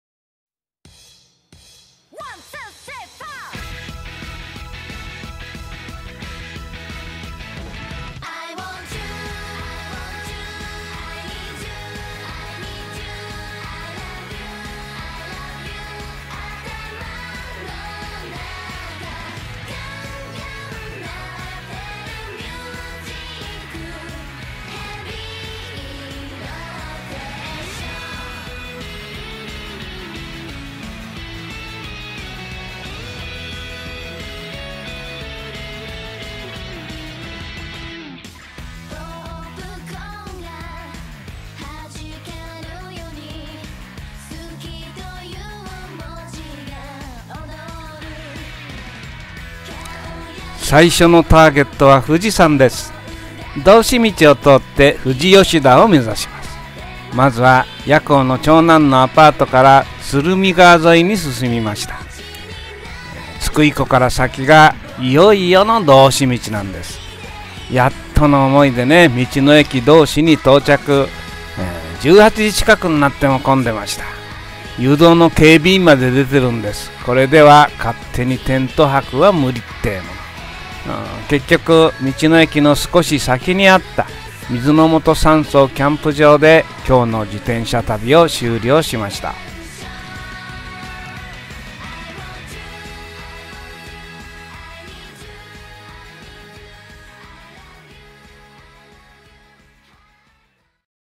百名山日本周遊人力旅富士南ア編1日目 炎天下の道志道
新章第12章　富士南ア編スタート
まずは富士山めざして道志道を行きました

7月18日(月) 晴れ時々曇り 第１日
やっぱり夏は暑いのだ！
自転車旅 走行距離85.69km 高度上昇1105m
矢向09:15-新横浜-長津田-町田-相模原-13:45津久井湖14:00-青野原-道志17:20-17:45道の駅どうし18:10-18:20水之元山荘キャンプ場
湯＝水之元山荘
泊＝水之元山荘キャンプ場